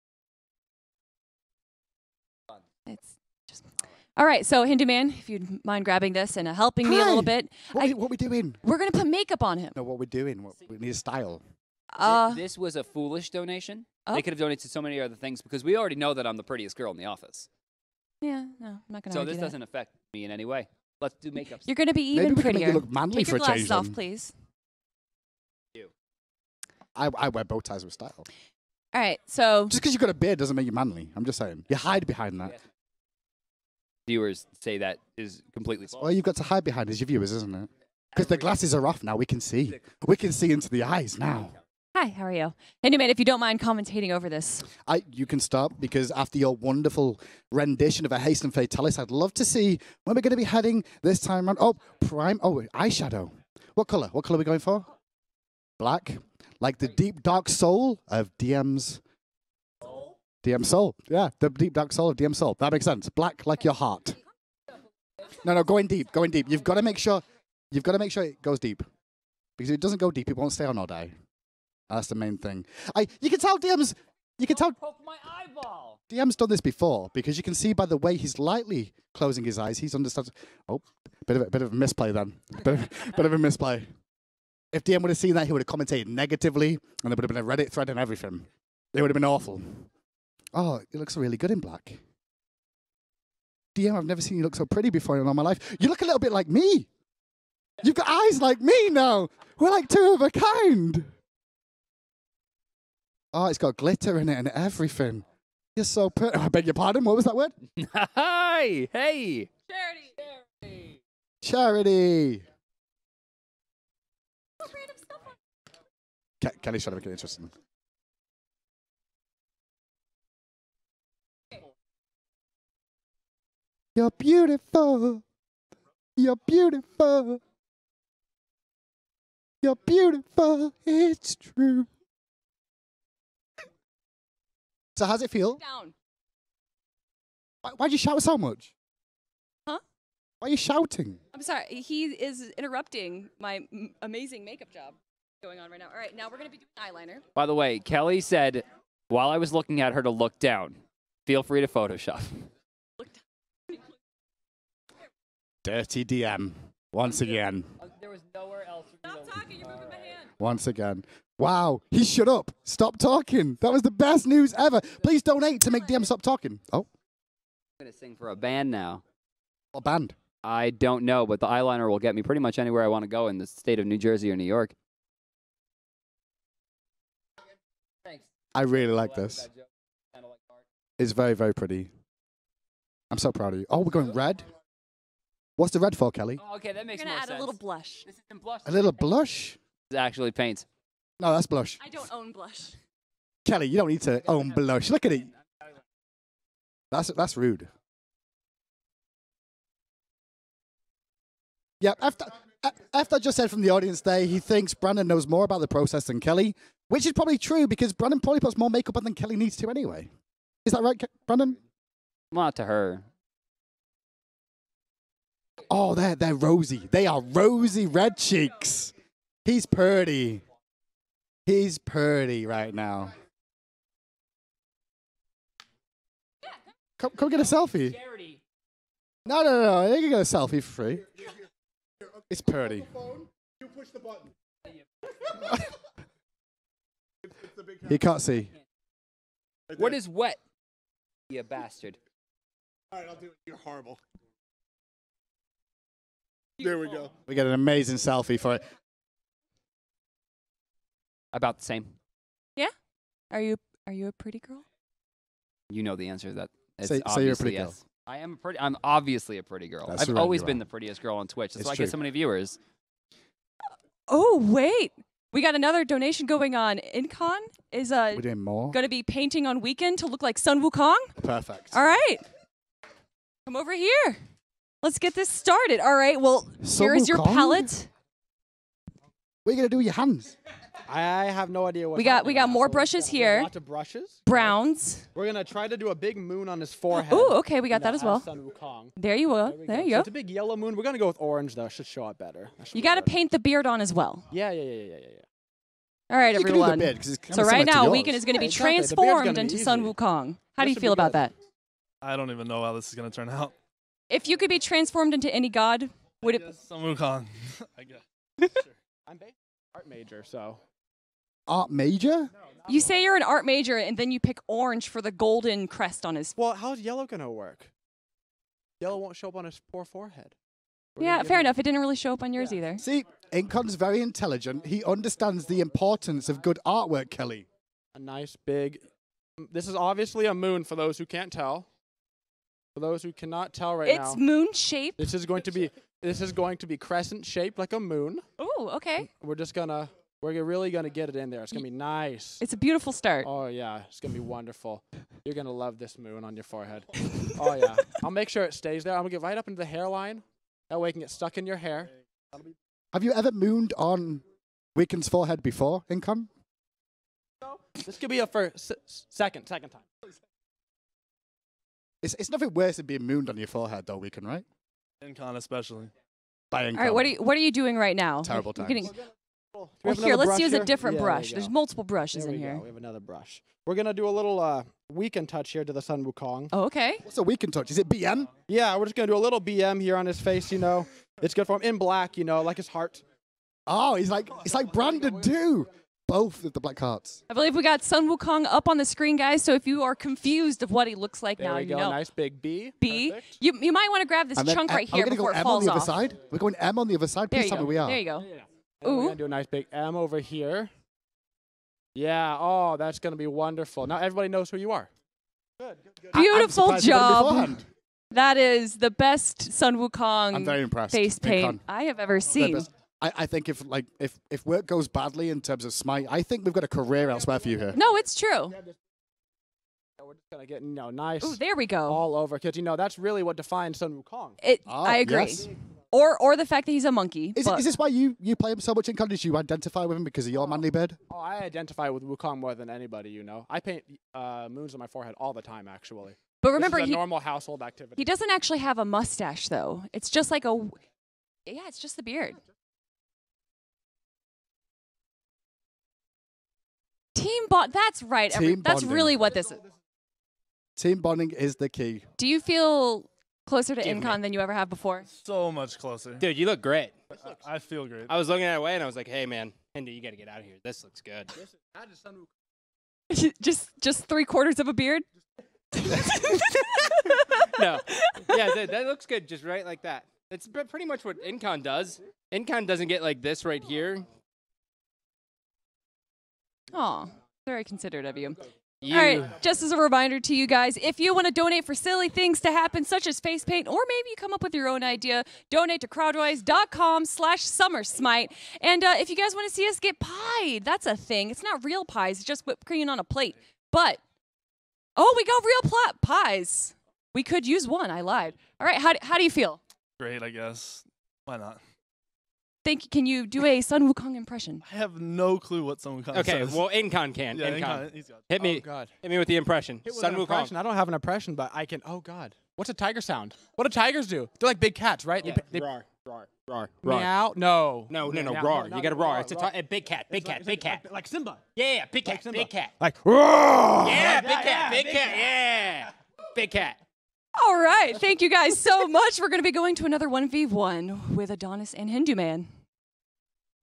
Summer Charity LAN - Diem makeover & Facepainting Weak3n
Hi-Rez Summer Fling Charity Stream to benefit the American Red Cross. (Friday June 26th, 2015)

00m00s - Diem Brandon gets a makeover!
04m30s - Weak3n gets painted by Incon.